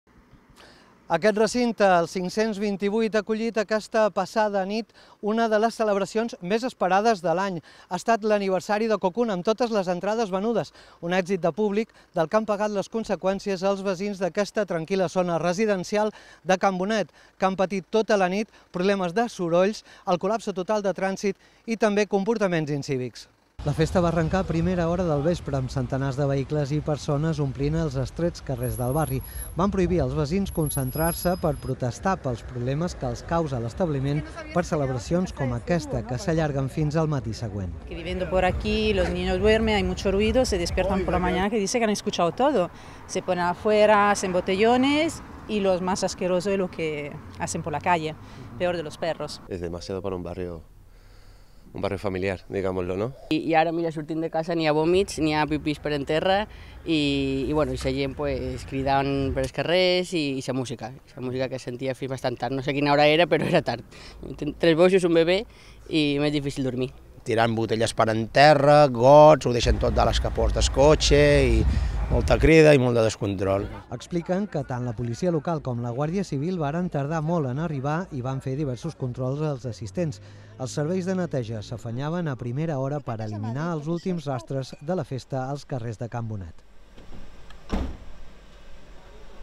Indignació a Can Bonet per una nova festa massiva
Els vesins de la barriada de Sant Antoni denuncien la brutícia i l'incivisme que han de patir